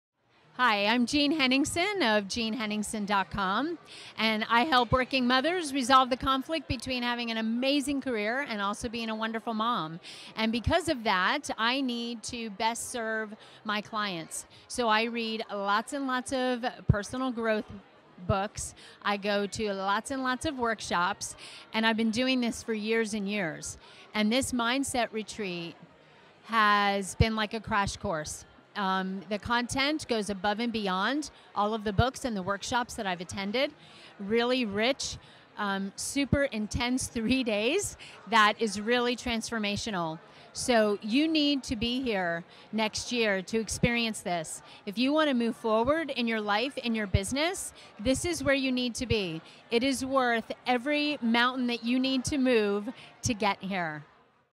Jeanne Henningsen at The Mindset Retreat 2013
Jeanne Henningsen speaks about her experience at the Mindset Retreat 2013 Live Event.